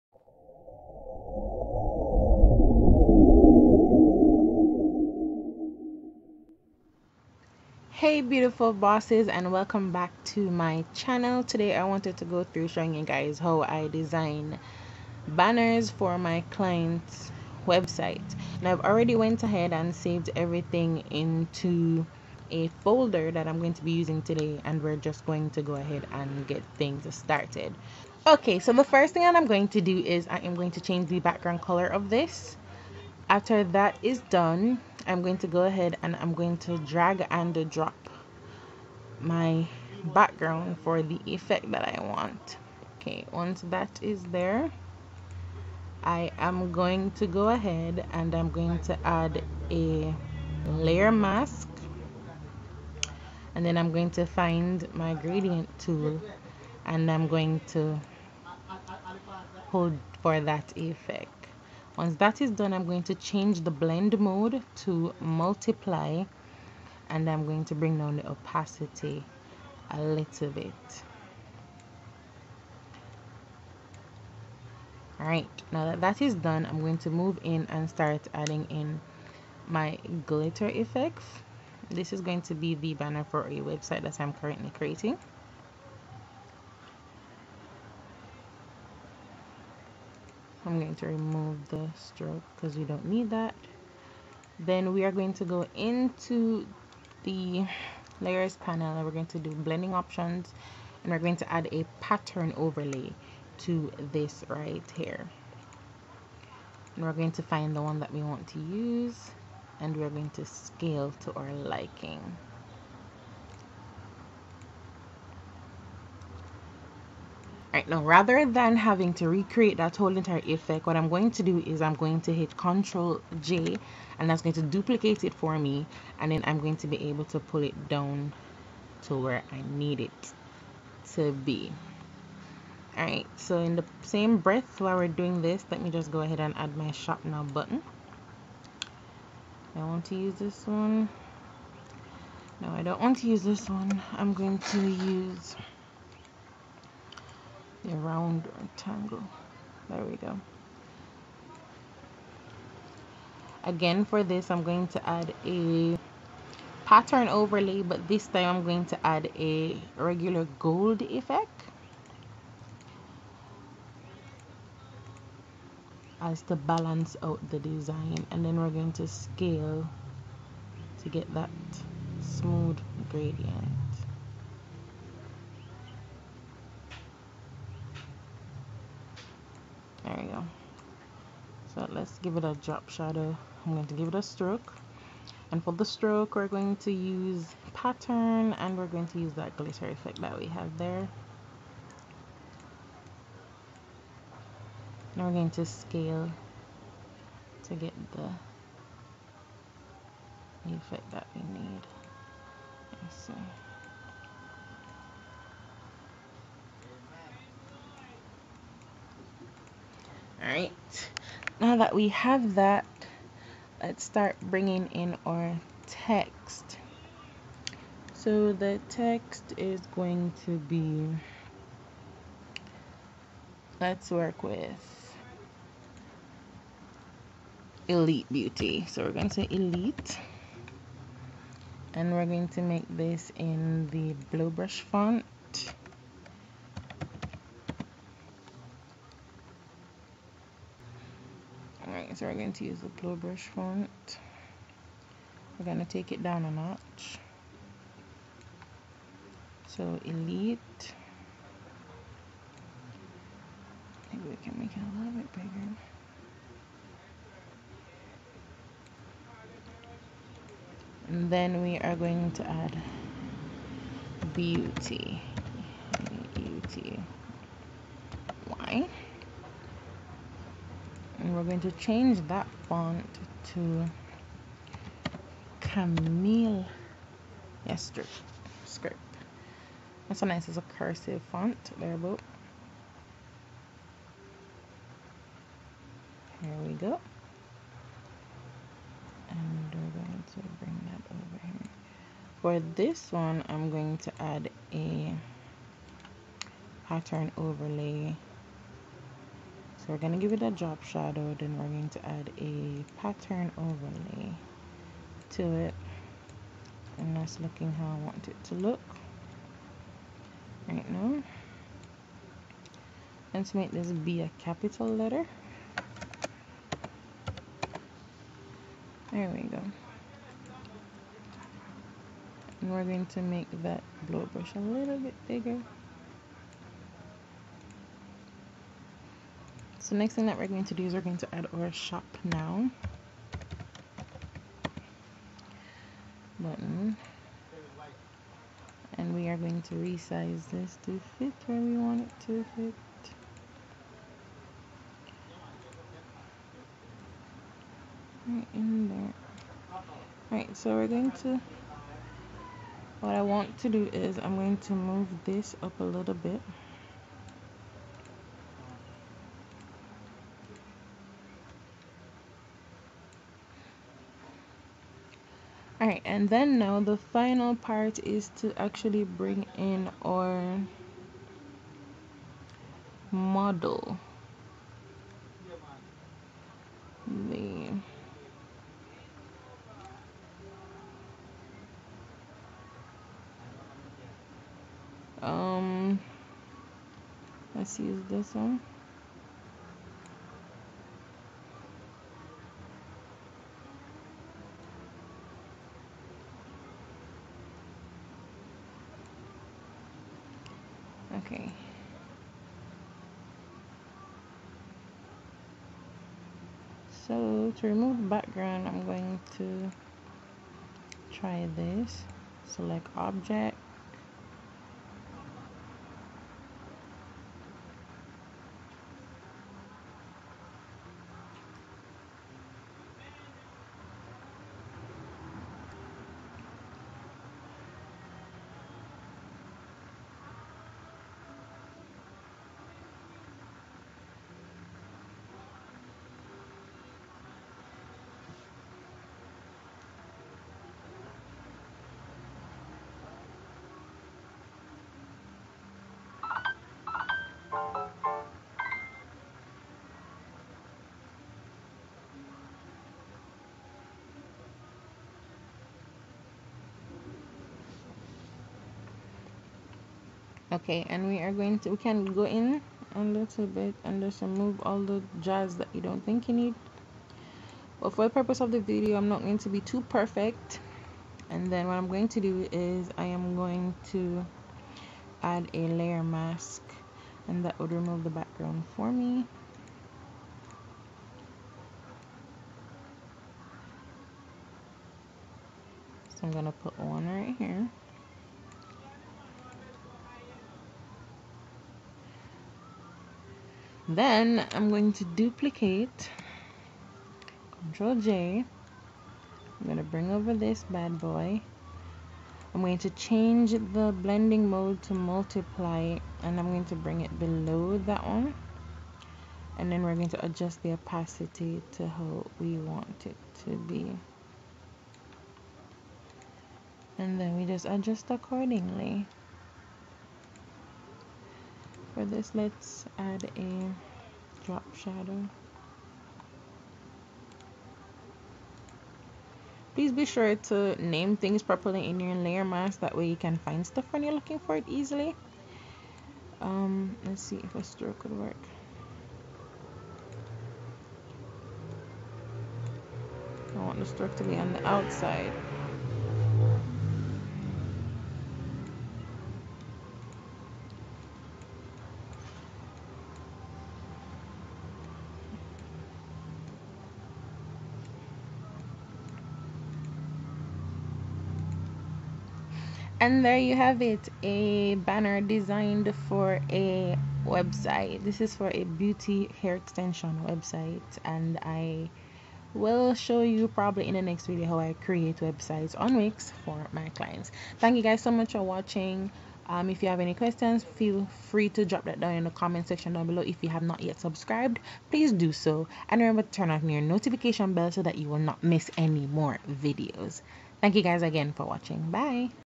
Photoshop tutorials - Create an Eye-catching Shopify Banner | Easy Shopify Banner With Photoshop
Photoshop tutorials - Create an Eye-catching Shopify Banner with Photoshop | Easy Shopify Banner With Photoshop - Hey, boss babes! If you are trying to build your own Wix or Shopify store and need a simple but pro way to do it then this is for you. Press play and I'll show you how to make a beautiful  hair store banner with Photoshop.